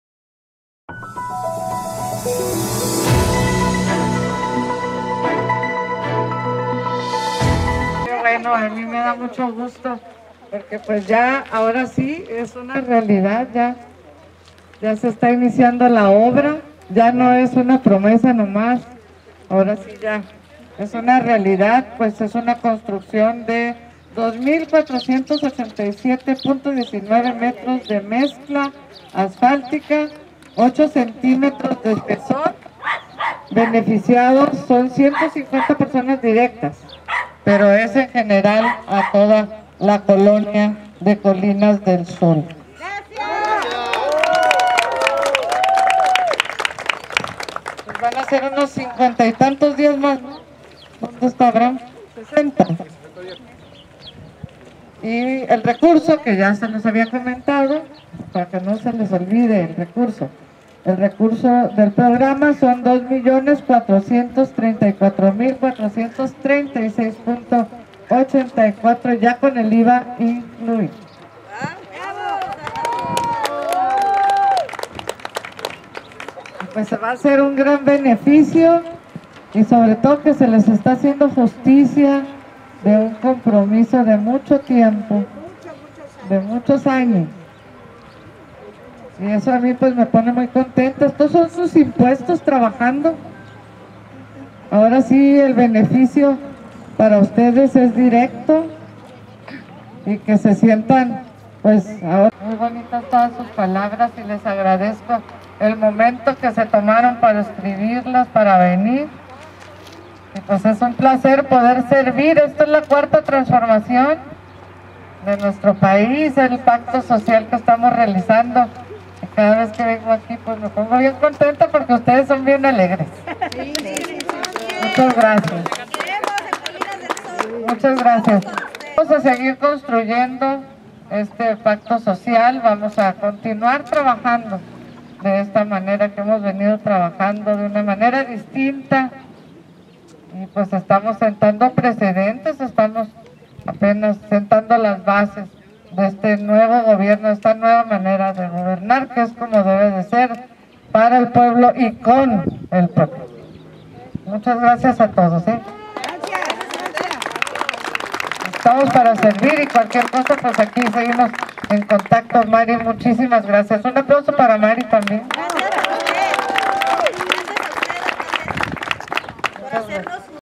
CNR Rosarito Pavimentan vialidad en colinas del sol
sin lugar a dudas unas de las colonias más necesitadas es la colonia colinas del sol en playas de rosarito, muchas de sus calles carecen de servicios básicos tales como pavimentación, drenaje, agua potable, alumbrado público etc, precisamente para atender las necesidades de los núcleos poblacionales más afectados se dio banderazo de inicio a la obra de pavimentación a las calles circuito febe y colina neptuno en la colonia colinas del sol, para esta obra se destinaron más de dos millones de pesos y se utilizaran materiales de la mejor calidad para evitar que la vialidad sufra desperfectos como baches y grietas que dificulten el transitar por esa vialidades, esta obra se está realizando con recurso propio del municipio es decir, lo recaudado mediante impuestos se destina a obras y mejoramiento de vialidades